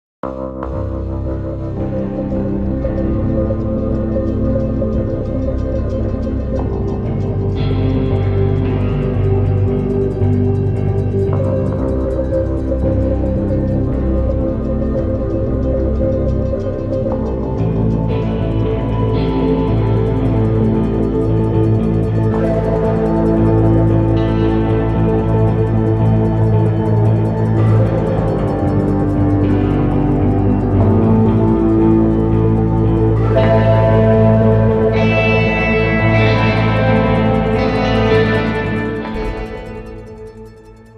Rare footage of King Vulture
The king vulture is a large bird found in Central and South America. It is a member of the New World vulture family Cathartidae. This vulture lives predominantly in tropical lowland forests stretching from southern Mexico to northern Argentina.